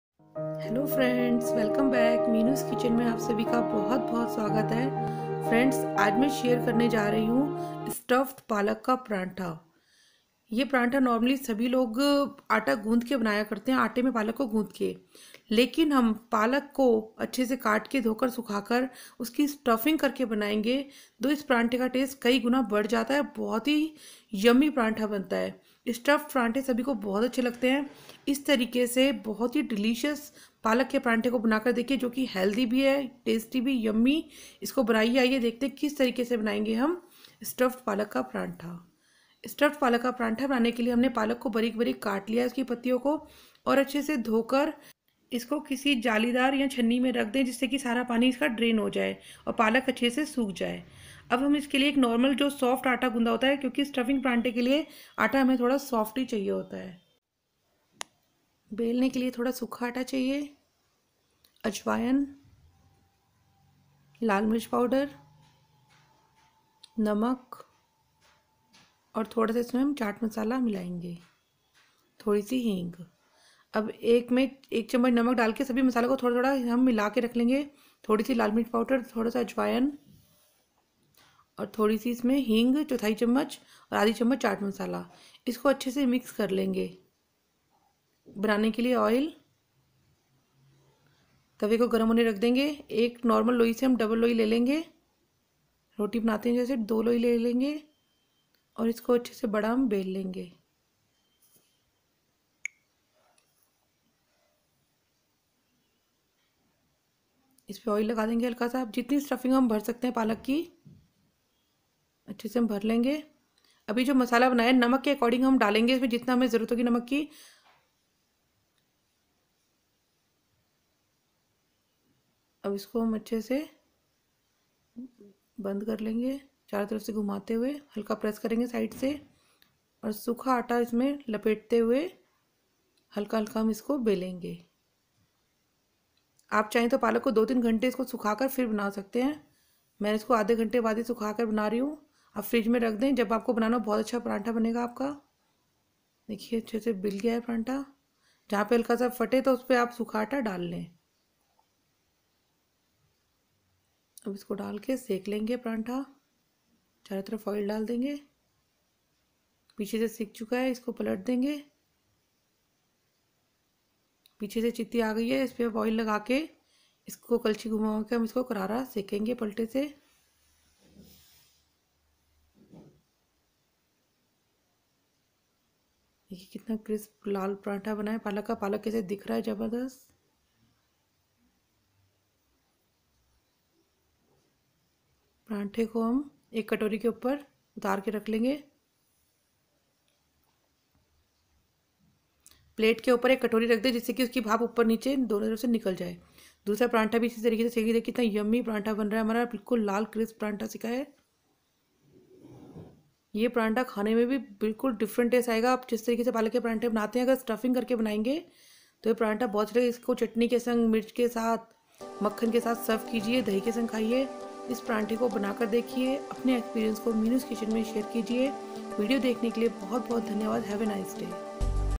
Stuffed Palak Bharva paratha ( इस तरह से पालक को भर कर बनाएंगे पराठा तो मजा आ जाएगा)
पालक का भरा हुआ पराठा था बहुत ही स्वादिष्ट होता है और सभी को बहुत पसंद आता है मेरा तो यह सबसे पसंदीदा पराठा है आप भी इस तरह से पराठा बनाइए बहुत ही अच्छा लगेगा
Stuffed Aloo Green Moti Mirch Recipe in hindi (आलू की भरवा मोटी हरी मिर्च मजेदार स्वाद)